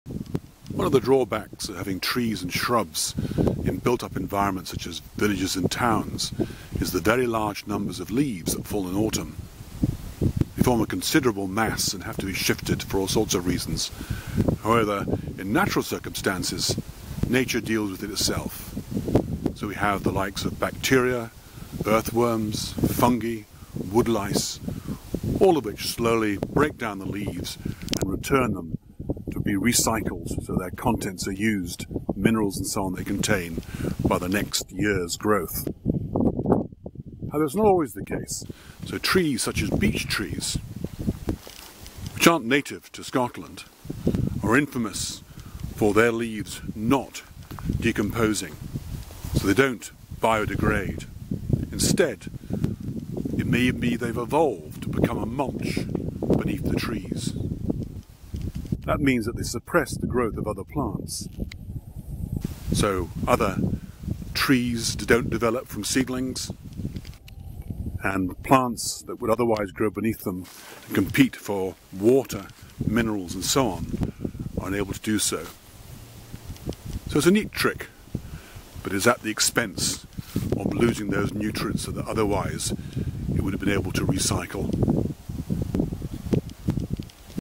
Nature Insights - Where do all the Autumn Leaves Go?
The fall and recycling of leaves in tree and shrub species with Beech given as an exception where the leaves act as a mulch that restricts the growth of competing species.